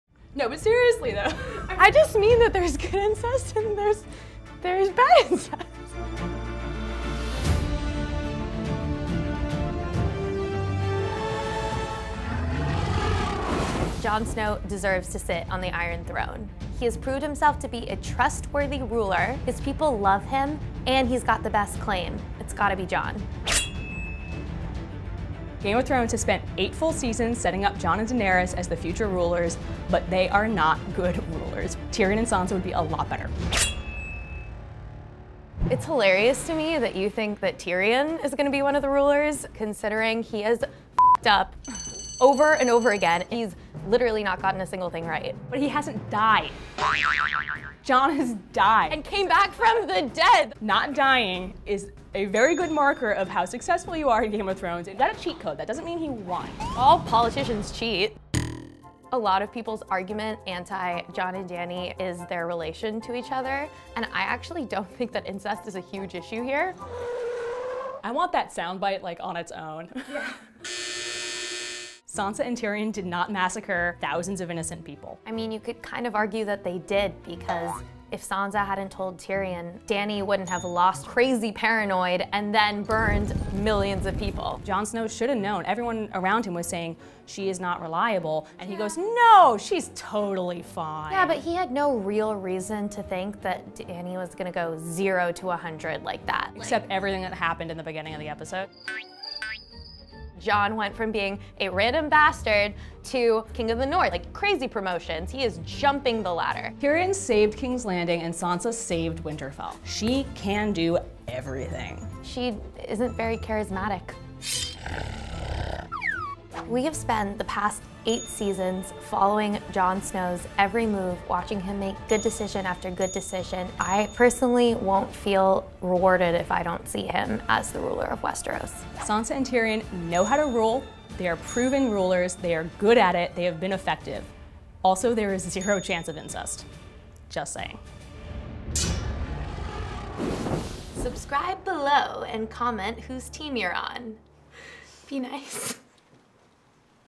Jon Snow and Daenerys would be TERRIBLE rulers!!! | Fight Me On This
Calling all GoT fans: what’s YOUR theory about who will end up on the Iron Throne?